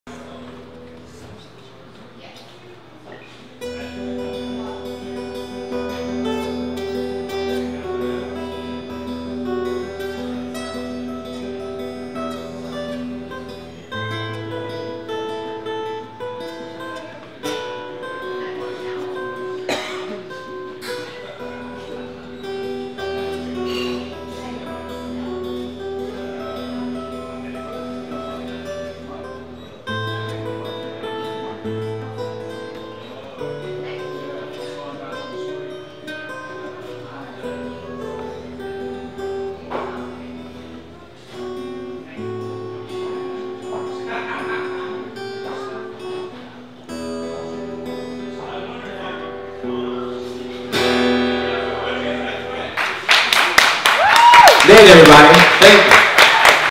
Nate Student Performance - Roots Bistro Open Mic - Instrumental Guitar Piece
Matt Graves Georgetown Guitar Instructors Student Nate performs a short instrumental piece at the Roots Bistro Open Mic on Wednesday April 1st, 2015.